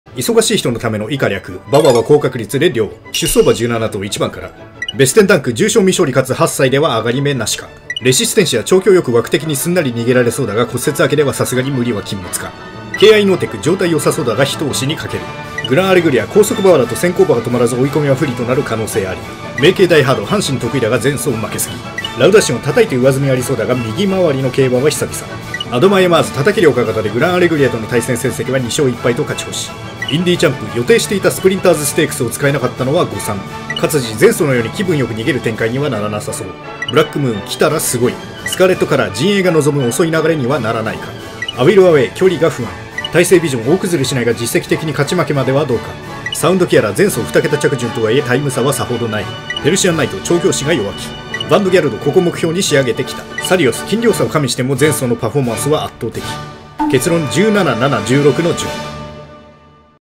【2020マイルチャンピオンシップ 枠順確定】忙しい人のための競馬予想（１分間全頭診断）
今年は阪神！短距離ＧＩ３連勝を狙うグランアレグリアや三冠馬コントレイルの好敵手サリオスなどが出走する集結する2020年マイルCSの予想を１分間にまとめました